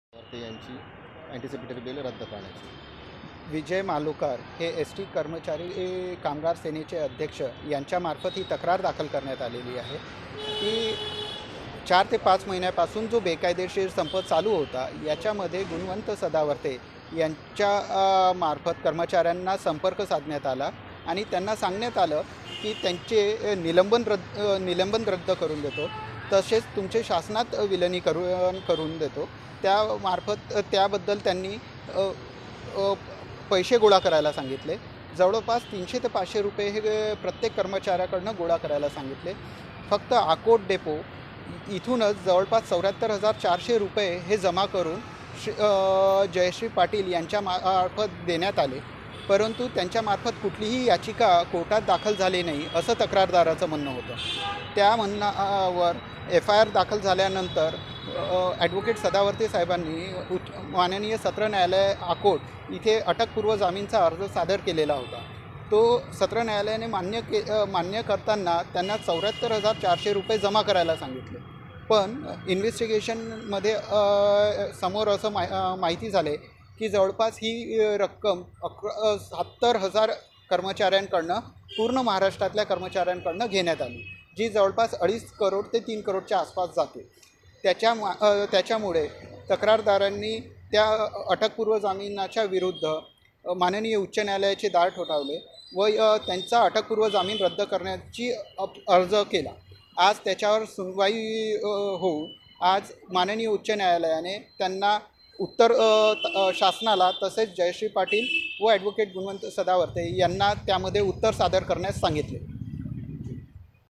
NAGPUR | ऍड गुणरत्न सदावर्ते यांना मिळालेला अटकपूर्व जामीन रद्द करण्यात यावा - आशिष फुले
ऍड गुणरत्न सदावर्ते यांना मिळालेला अटकपूर्व जामीन रद्द करण्यात यावा यासाठी मुंबई उच्च न्यायालयाच्या  नागपूर खंडपीठात याचिका दाखल करण्यात आली .. याचिका स्वीकारून हाय कोर्टाने सदावर्ते व राज्य सरकारला नोटीस जारी केली . एसटी कर्मचाऱ्यांकडून पैसे गोळा करून पैशाचा अपहार केल्याप्रकरणी अकोला जिल्ह्याच्या आकोट पोलिस स्टेशन मध्ये सदावर्ते यांच्याविरुद्ध गुन्हा दाखल करण्यात आला होता . दाखल गुन्ह्यात सदावर्ते यांना आकोट सत्र न्यायालयाने अटकपूर्व जमीन मंजूर केला होता . हा अटकपूर्व जमीन रद्द करण्यात यावा यासाठी अकोला येथील विजय मालोकर यांनी नागपूर खंडपीठात याचिका दाखल केली आहे ..

बाईट - आशिष फुले , याचिका कर्ताचे वकील